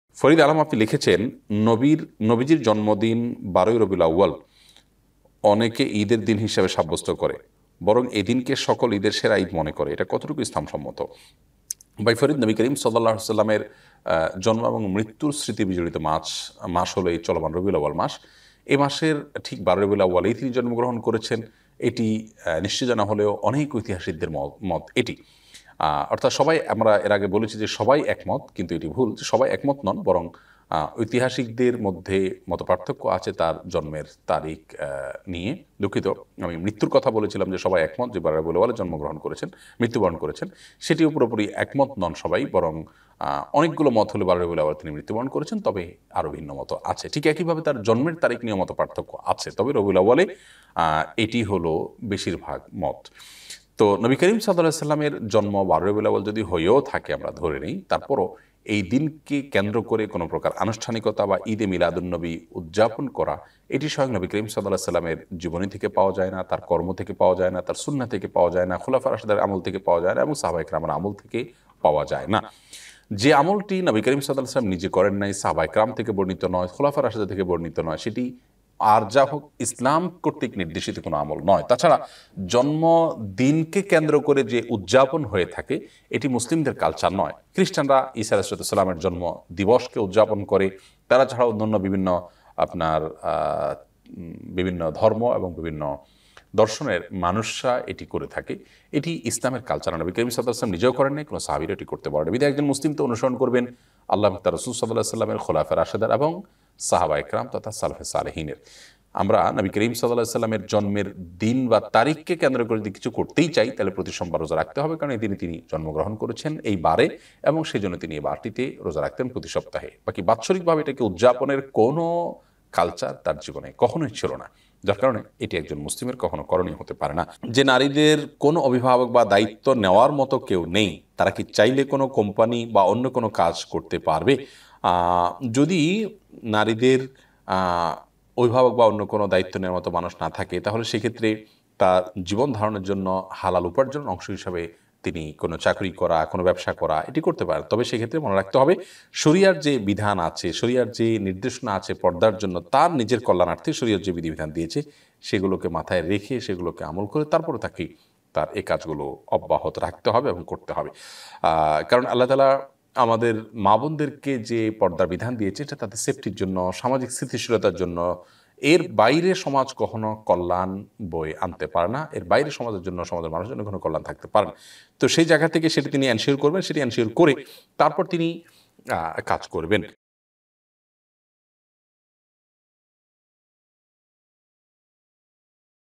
১২ রবিউল আউয়ালকে কি শ্রেষ্ঠ ঈদ বলা যাবে?
১২ রবিউল আউয়ালকে কি শ্রেষ্ঠ ঈদ বলা যাবে? ইসলামী প্রশ্নোত্তর II Sheikh ahmadullah live